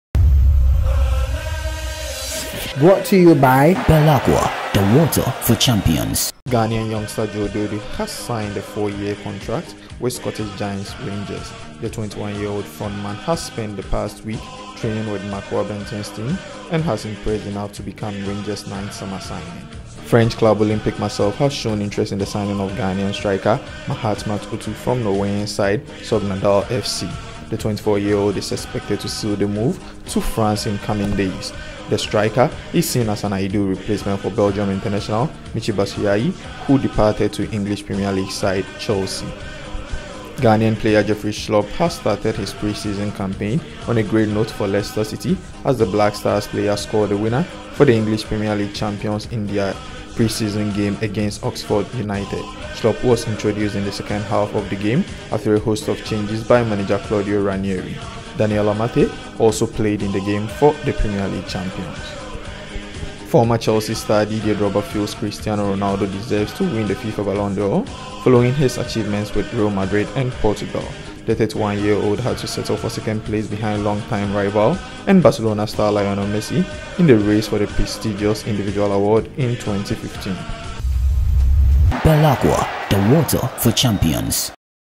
Pulse Sport News on the Go 20th July, 2016
Rangers sign Ghana youngster Joe Dodoo
Marseille interested in Mahatma Otoo
Jeffrey Schlupp scores winner in pre-season for Premier League champions
Drogba backs Cristiano Ronaldo for Ballon d’Or